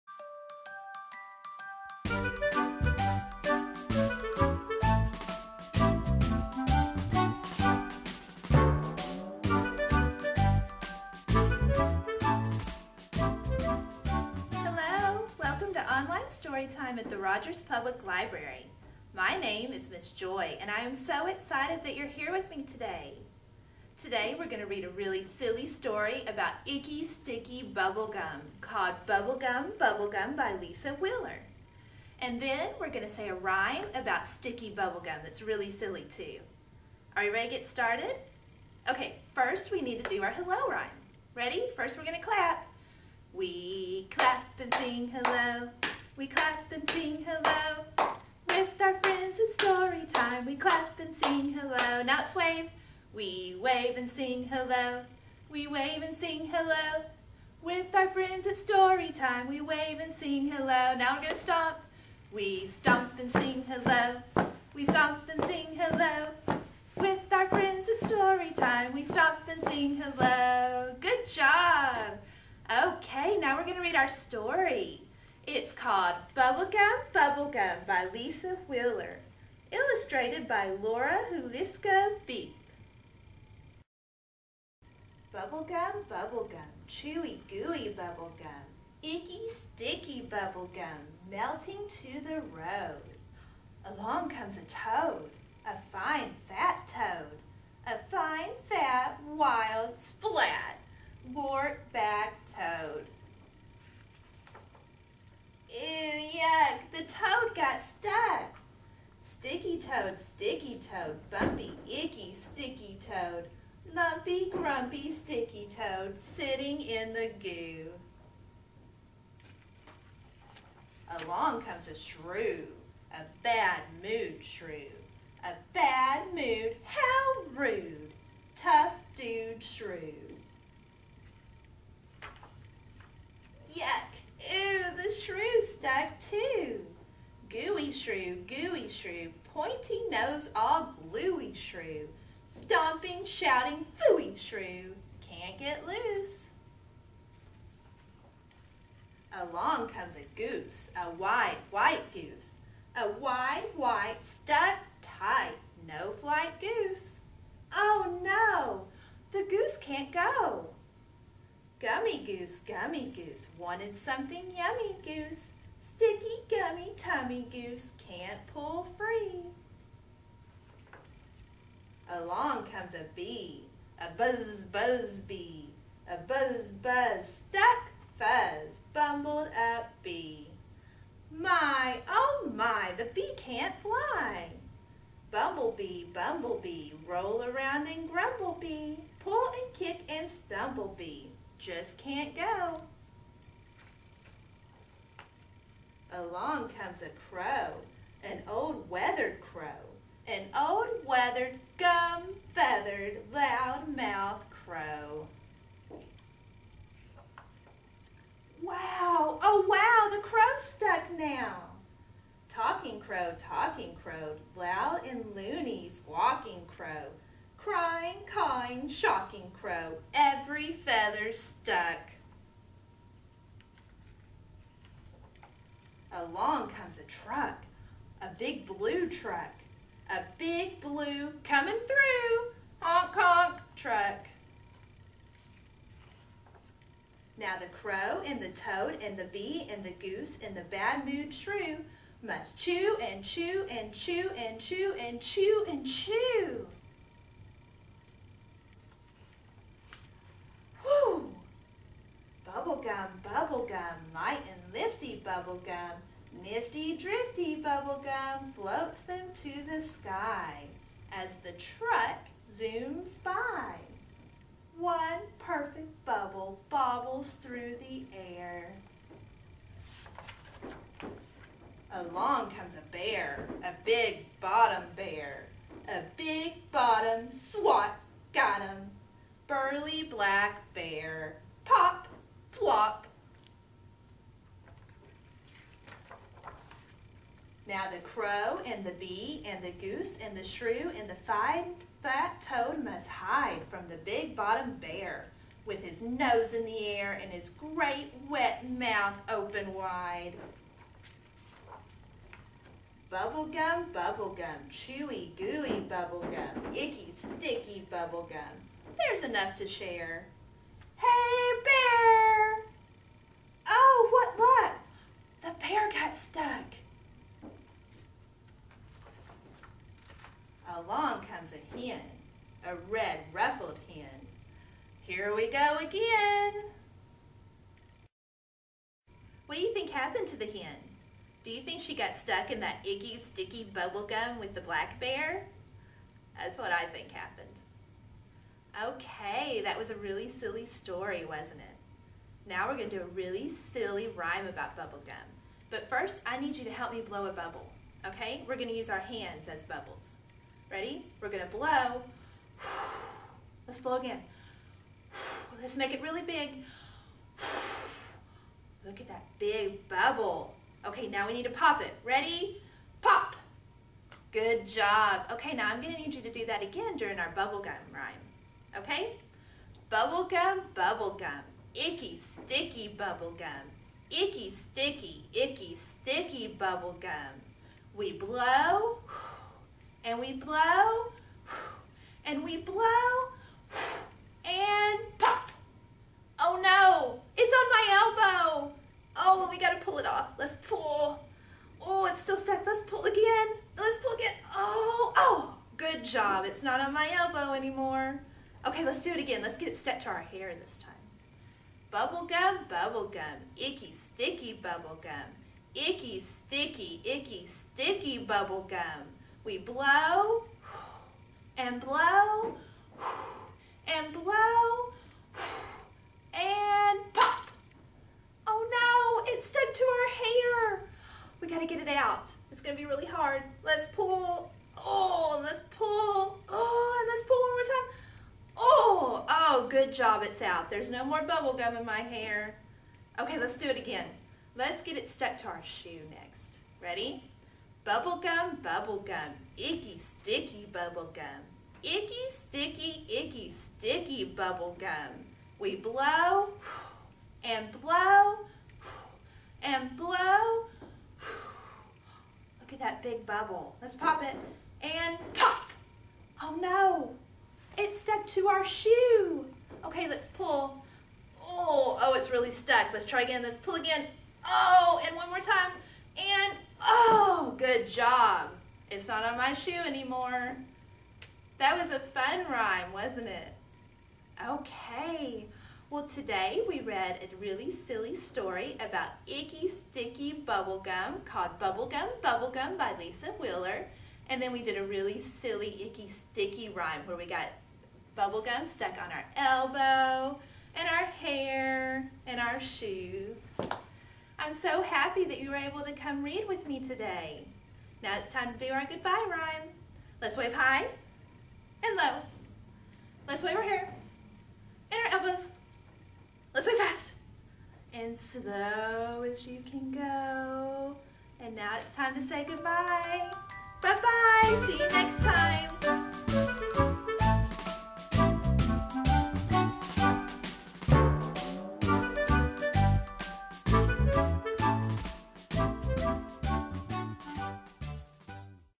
Bubble Gum, Bubble Gum Online Storytime with Miss Joy
Join Miss Joy for some bubble gum fun as she reads "Bubble Gum, Bubble Gum" by Lisa Wheeler, illustrated by Laura Huliska-Beith.